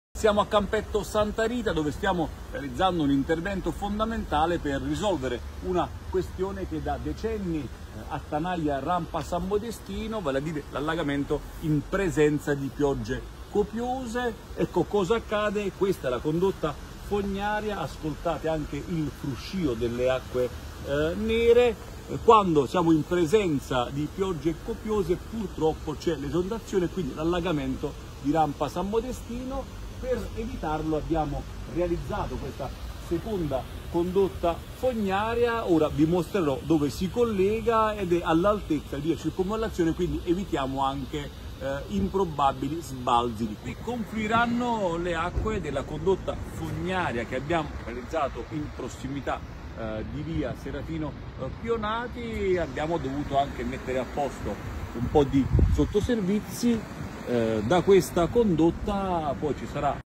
Avellino, Festa: “Questo che abbiamo realizzato stamattina è un intervento atteso da decenni”.
di Marco Iandolo -
(Tv7 Avellino) - Stop agli allagamenti di Rampa San Modestino. Qui, a Campetto Santa Rita, abbiamo realizzato una seconda condotta fognaria, che si allaccerà al sistema all’altezza di via Circumvallazione.
E’ un intervento atteso da decenni. Ogni qual volta le piogge erano copiose, le acque nere esondavano in strada. Così sistemiamo i sottoservizi e scriviamo la parola fine su una questione che nessuna amministrazione aveva mai affrontato.